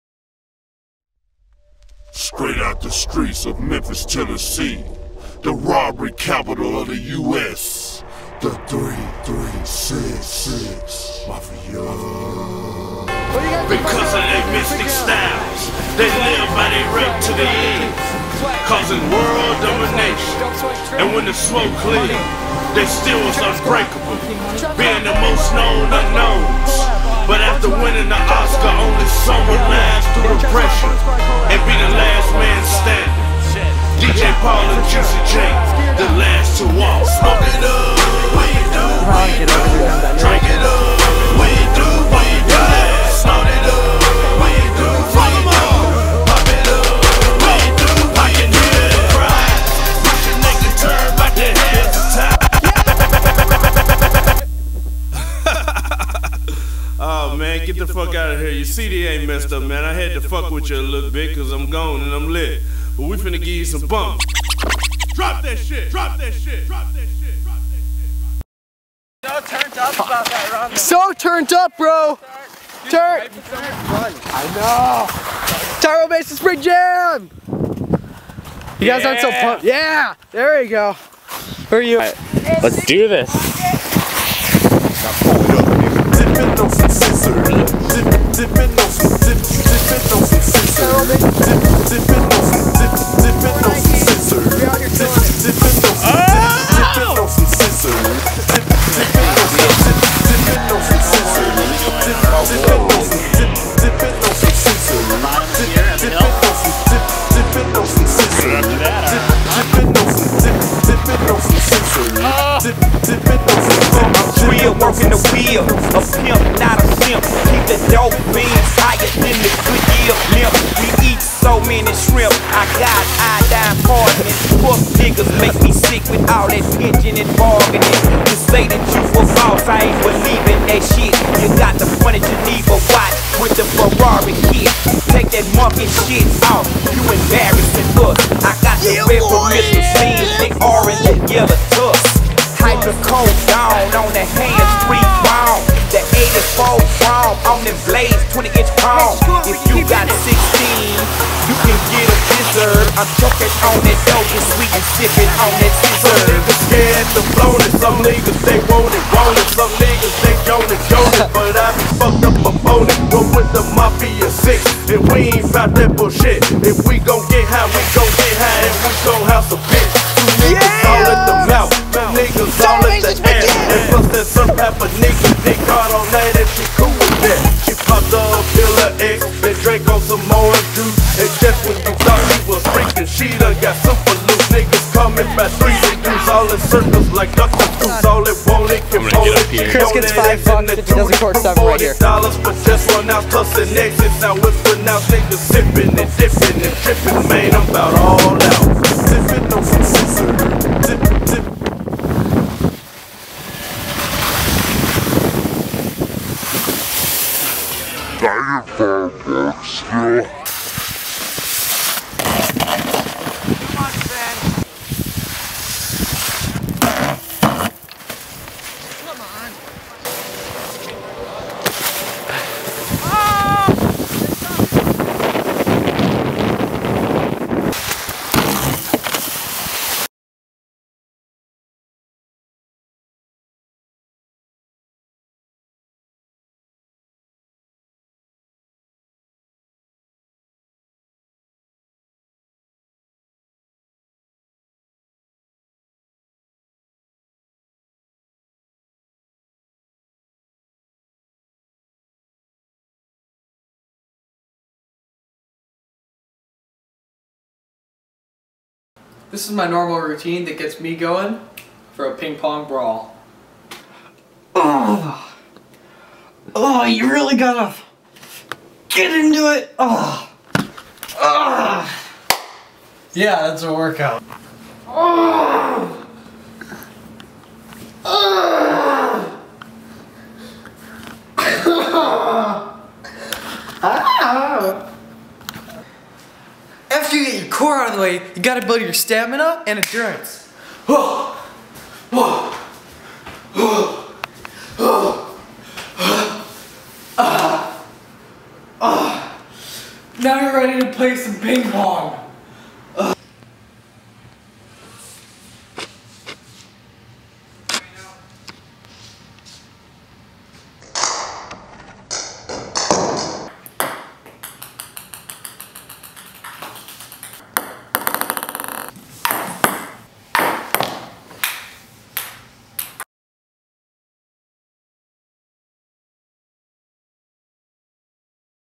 Lemur$ $ip the $cissor$
don't worry the editing is bad on purpose. Lemurs sip codeine.